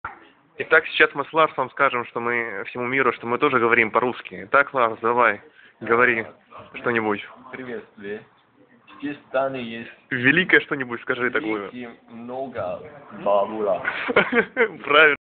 We speak russian in Denmark!
Danish friend Lars talk about main thing in Denmark in russian...